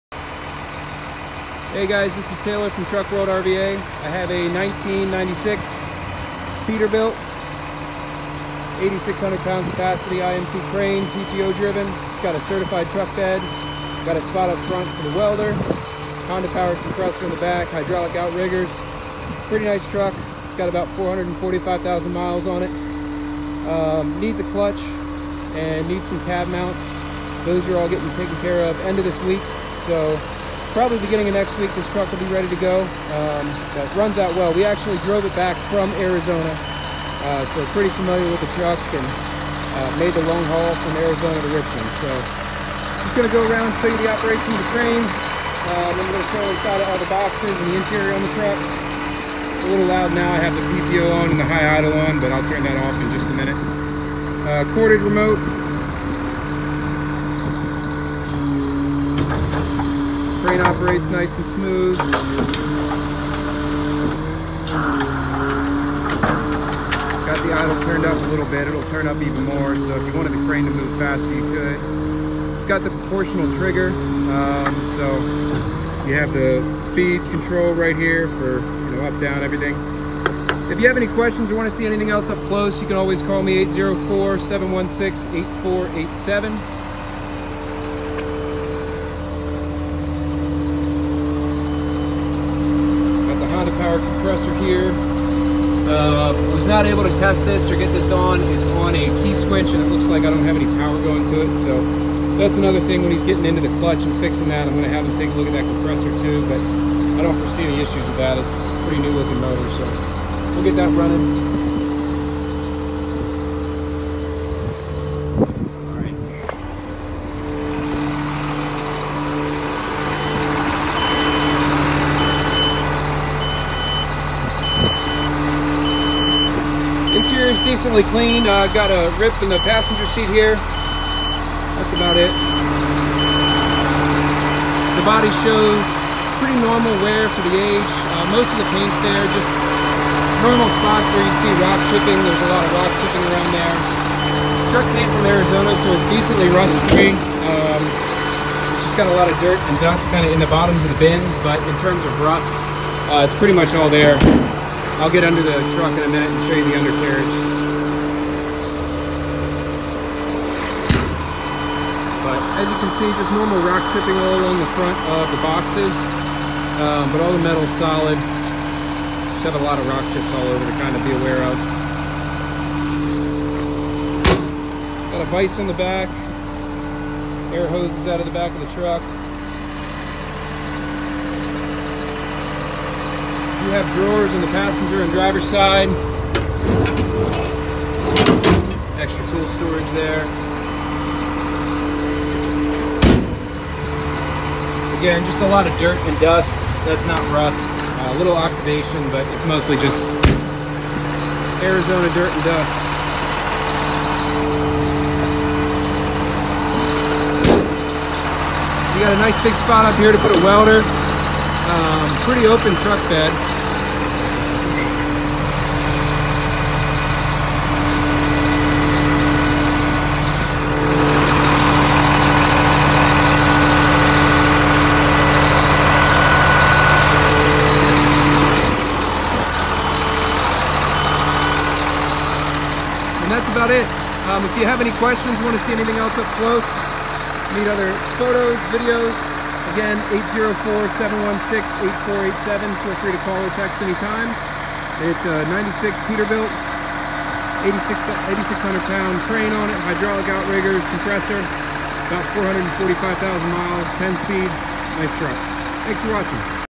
1998 Peterbilt 335, 455k Miles, 10Speed, 14ft Certified Truck Bed, IMT 8,600# Crane
Thanks for checking out this 98 Pete coming to us from Arizona!

455k miles, 10speed, driven here from AZ to Richmond. Runs well. We are putting a new clutch in this truck, as well as repairing some other minor issues. Should be ready in 4/21

IMT PTO driven crane with a 8,600# maximum lifting capacity. Hydraulic outriggers. Large Honda powered air compressor.

Rust free truck.

Thanks!